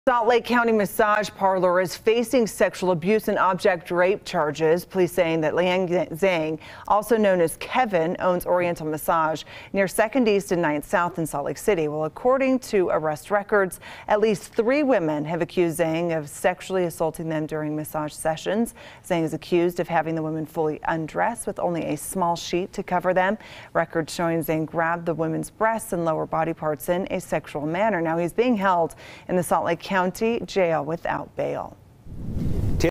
Massage Parlor Sex Abuse
A massage parlor owner has been arrested on charges of sexual abuse of his clients.